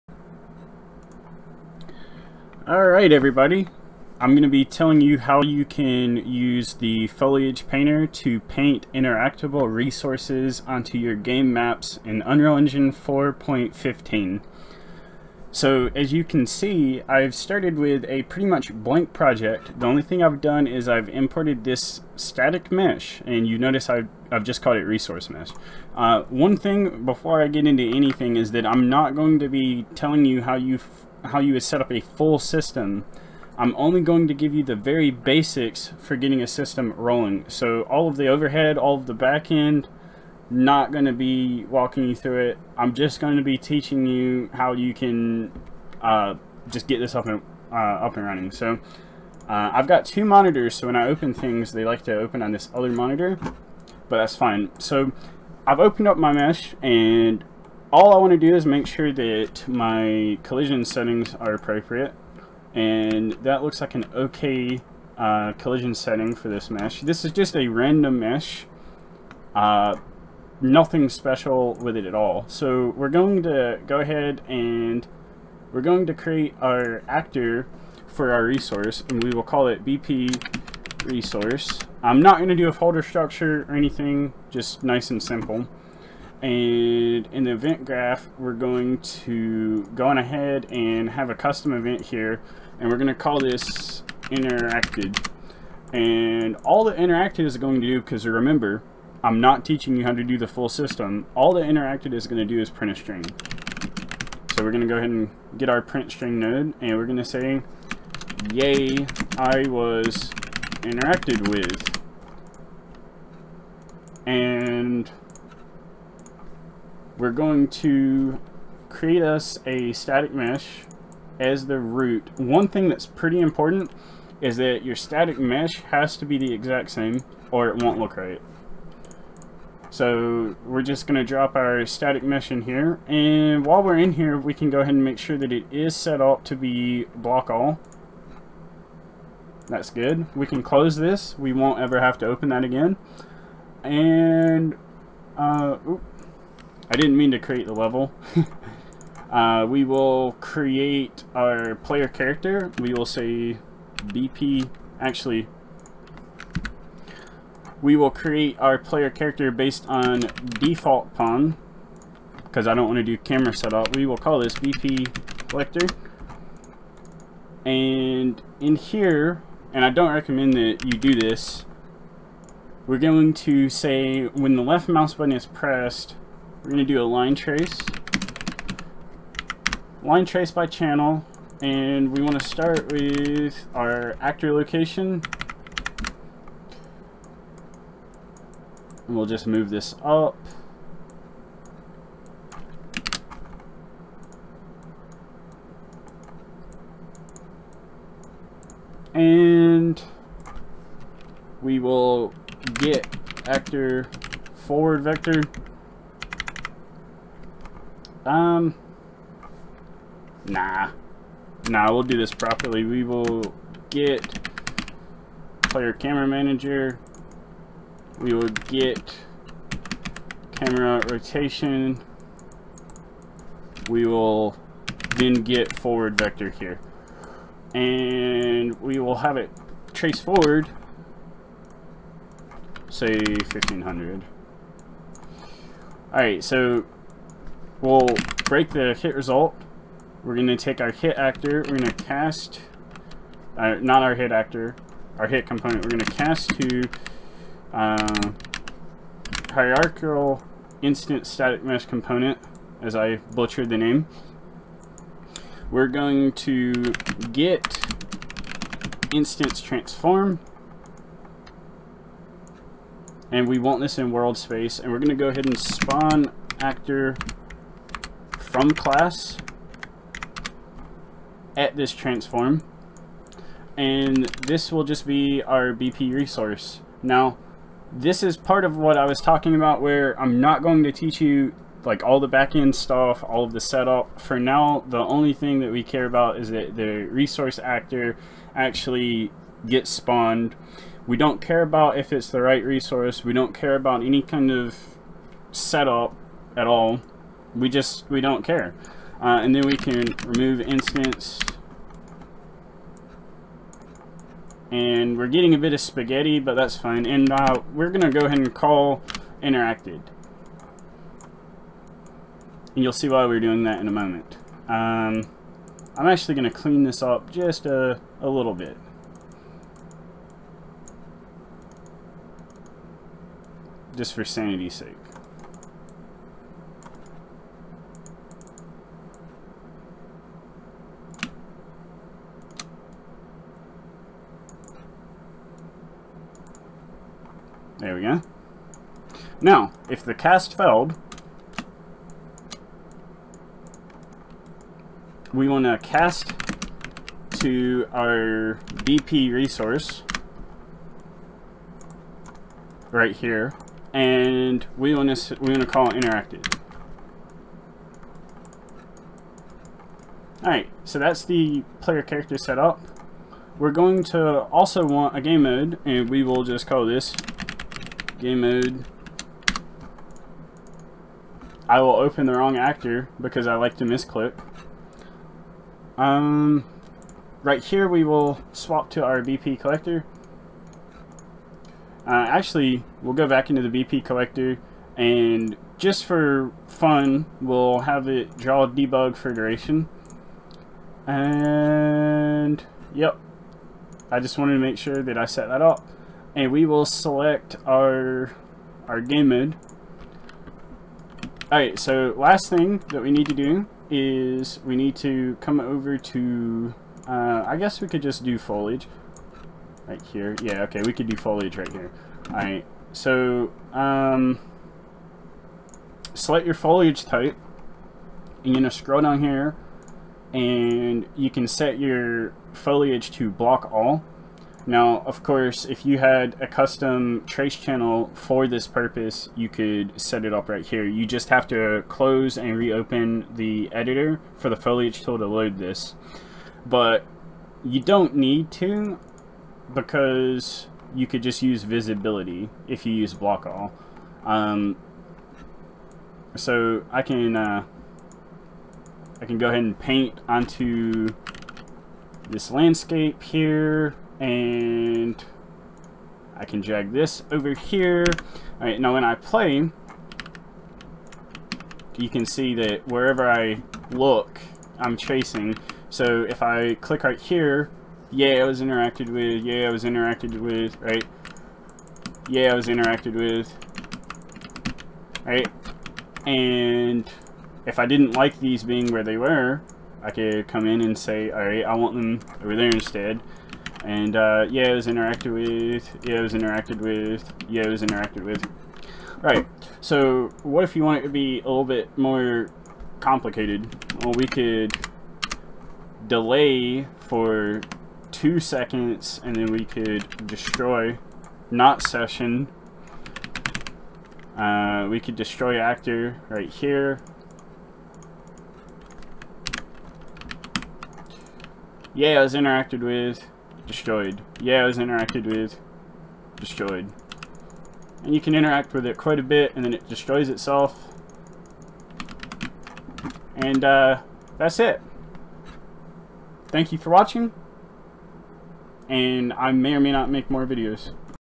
How to create instanced foliage resource actors in UE4 4.15 BP Only
For the more text-inclined, I made a post on my